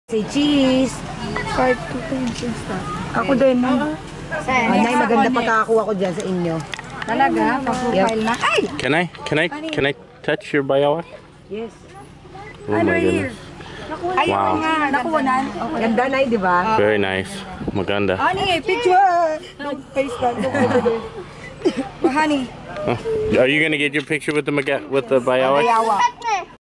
Bayowak kamoto dragon camoto .MP4
Lizzards native jungle girls present their pet dragons